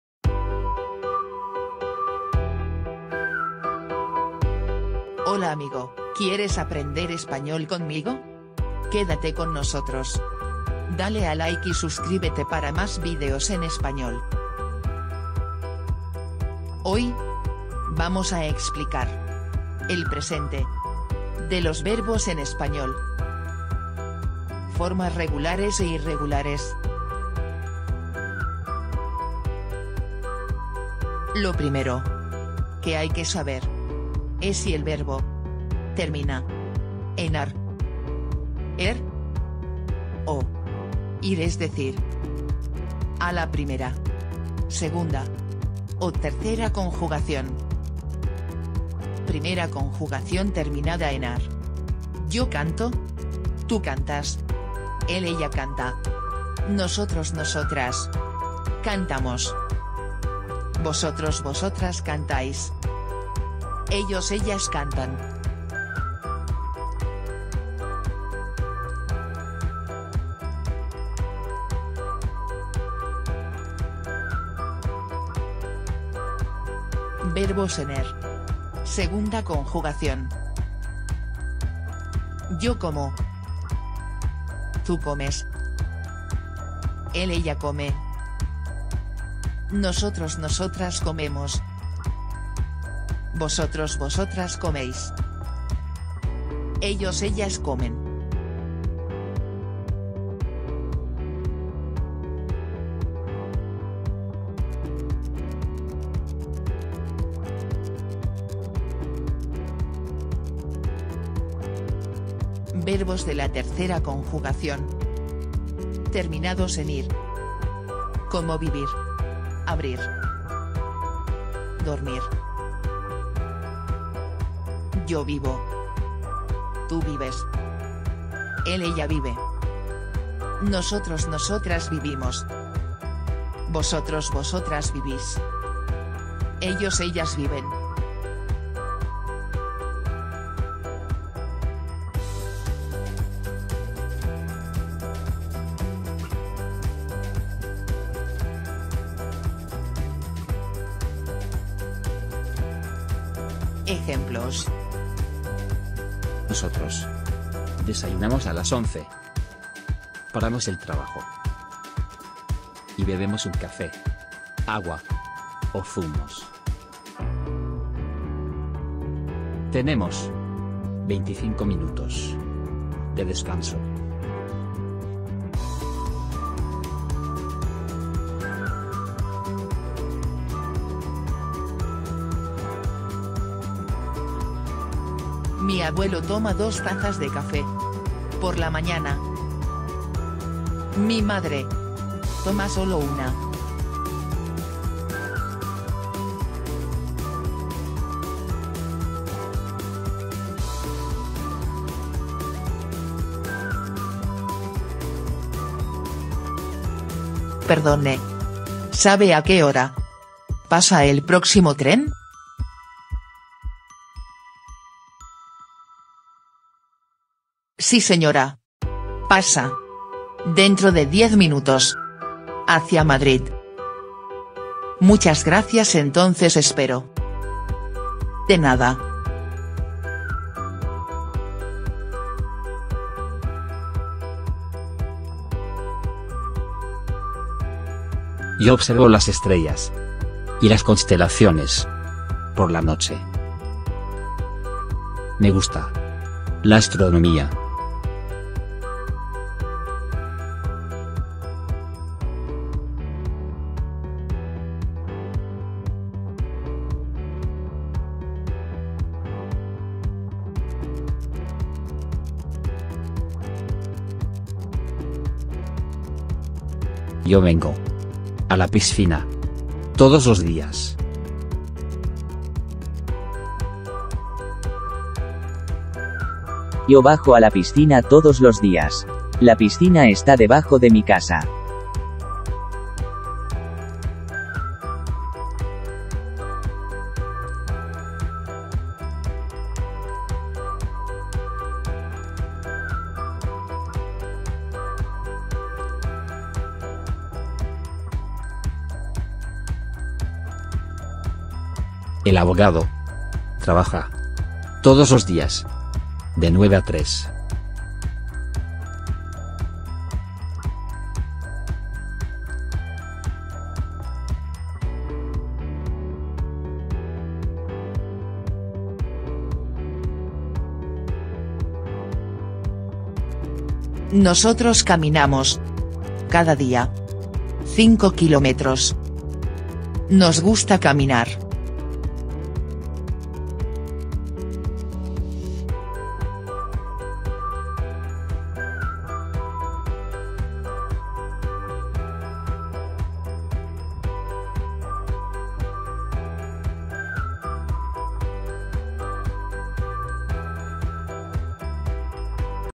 Aprende español básico Presente verbos y ejemplos
Aquí otro video sobre español .Presente de los verbos regulares.
Aprende español